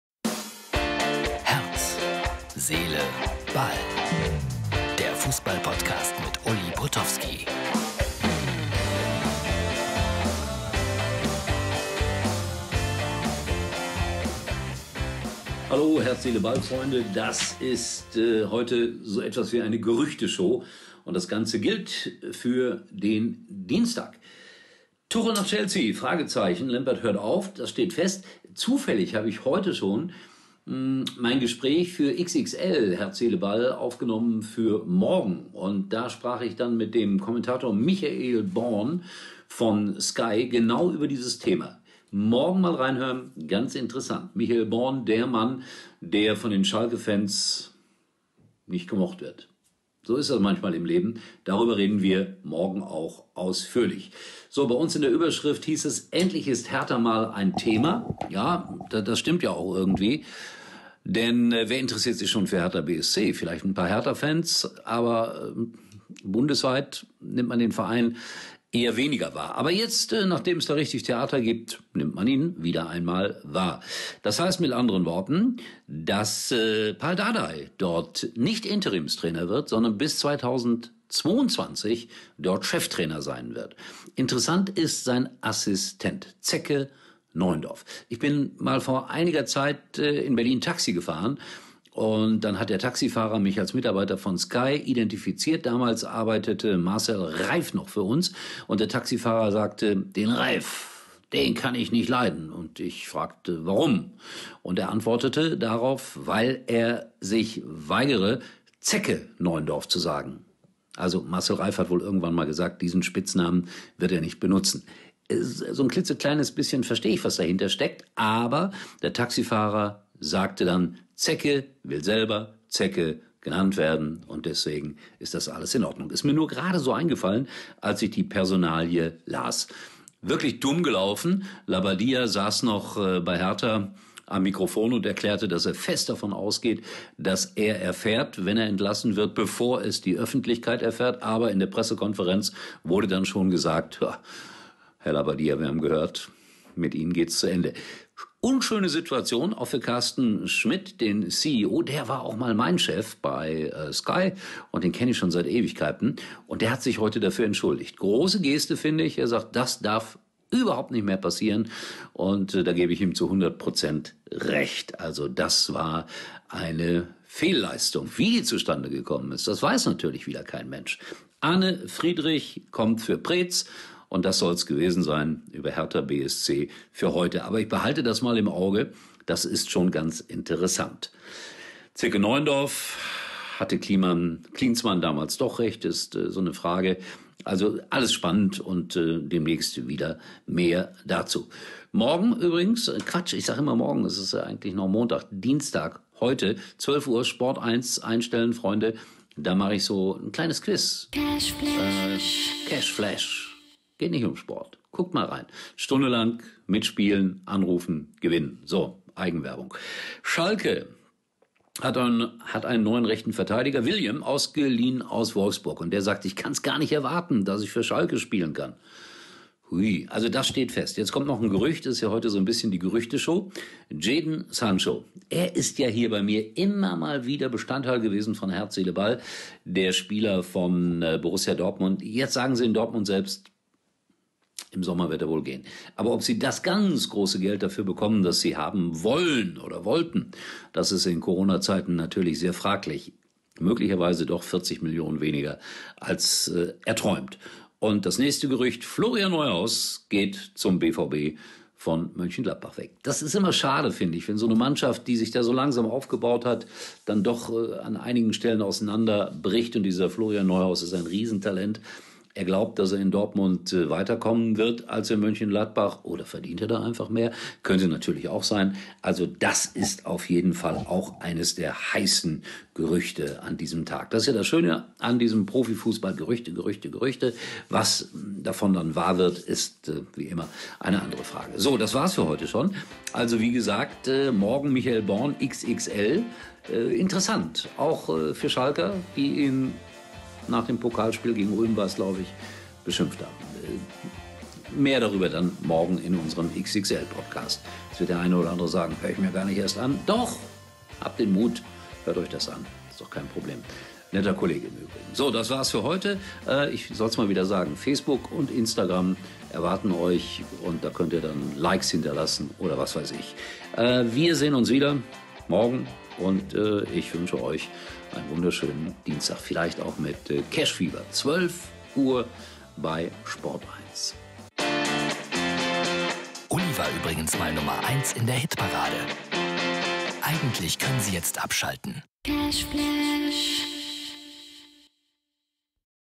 Herz • Seele • Ball - Folge 527 - Herz Seele Ball - Ulli Potofskis täglicher Fußballpodcast
Tuchel zu Chelsea
Hertha endlich mal ein Thema - und
noch ein neuer für Schalke 0:4